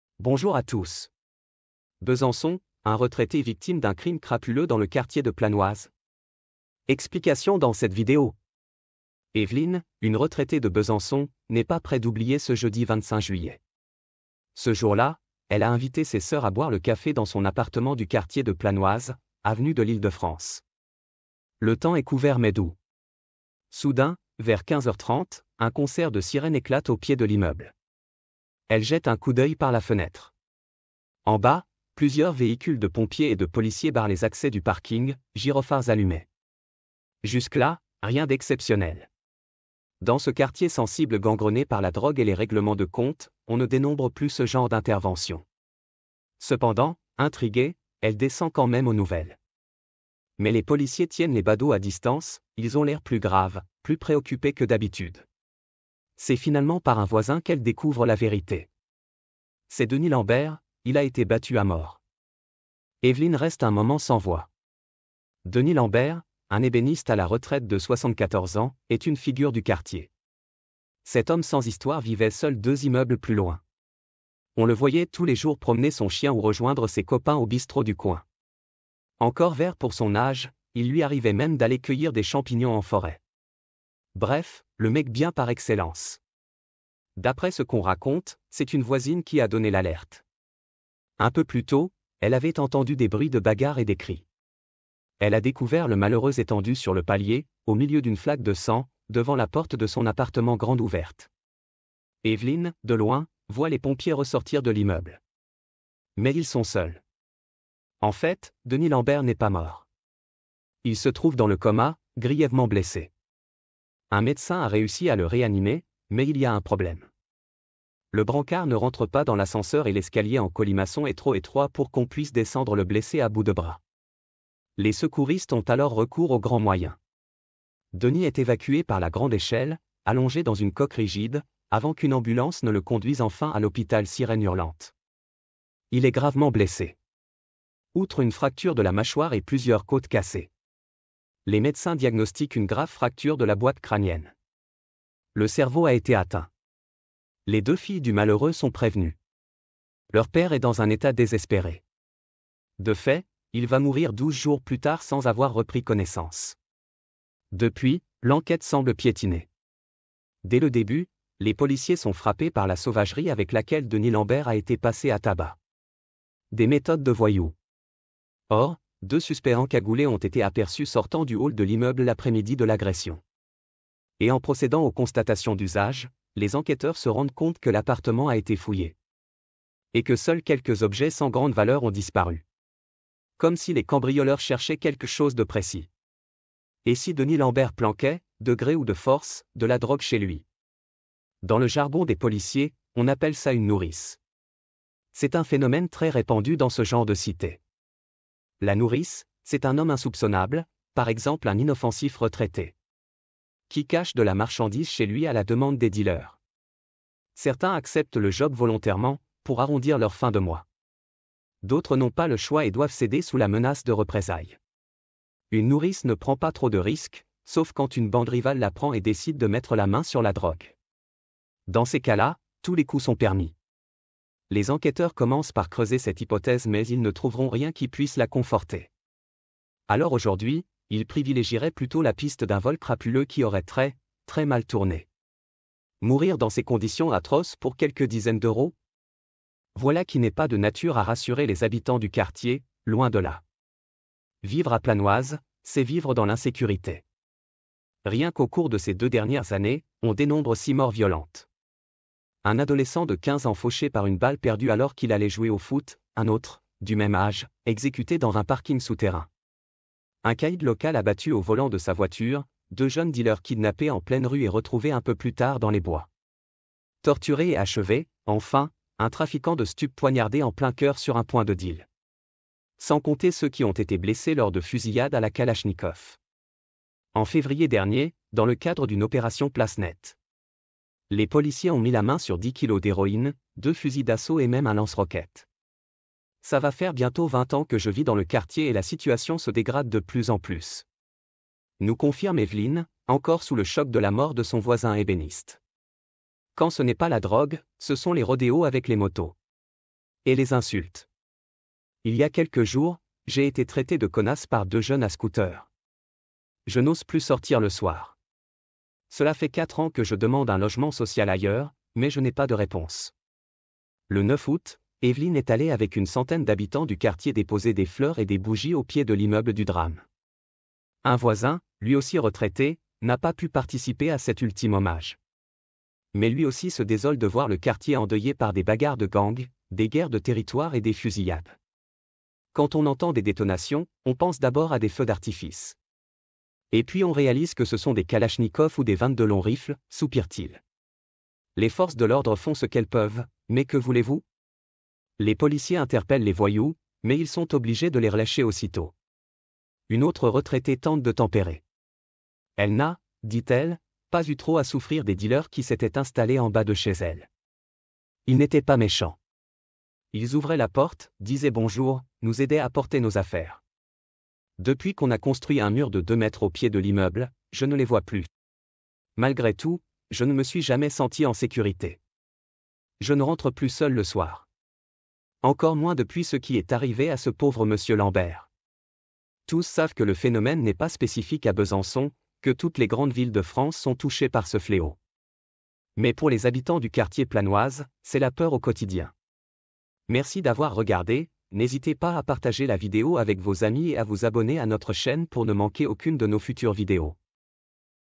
Besançon : Un retraité victime d’un crime crapuleux dans le quartier de Planoise.
Une enquête de Sixtine Leppert, avec Christophe Guerra.